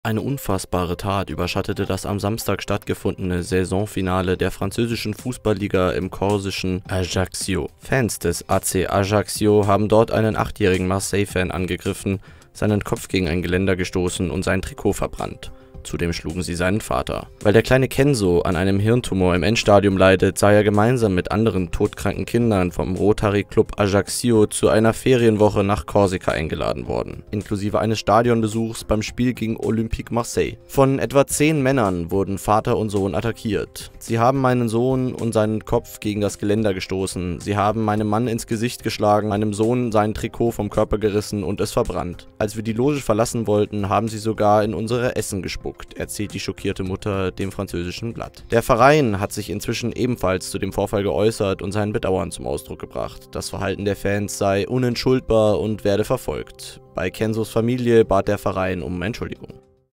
Fußballfans greifen Jungen mit Hirntumor an und verbrennen sein Trikot
Eine unfassbare Tat überschattet das am Samstag stattgefundene Saisonfinale der französischen Fußballliga im korsischen Ajaccio.

Fans des AC Ajaccio haben dort einen achtjährigen Marseille-Fan angegriffen, seinen Kopf gegen ein Geländer gestoßen und sein Trikot verbrannt. Zudem schlugen sie seinen Vater.
Weil der kleine Kenzo an einem Hirntumor im Endstadium leidet, sei er gemeinsam mit anderen todkranken Kindern vom Rotary Club Ajaccio zu einer Ferienwoche nach Korsika eingeladen worden. Inklusive eines Stadionbesuchs beim Spiel gegen Olympique Marseille.
Von etwa zehn Männer wurden Vater und Sohn attackiert. „Sie haben meinen Sohn und seinen Kopf gegen das Geländer gestoßen. Sie haben meinem Mann ins Gesicht geschlagen, meinem Sohn sein Trikot vom Körper gerissen und es verbrannt. Als wir die Loge verlassen wollten, haben sie sogar in unser Essen gespuckt“, erzählt die schockierte Mutter dem französischen Blatt.
Der Verein hat sich inzwischen ebenfalls zu dem Vorfall geäußert und sein Bedauern zum Ausdruck gebracht. Das Verhalten der Fans sei „unentschuldbar“ und werde verfolgt. Bei Kenzos Familie bat der Verein um Entschuldigung.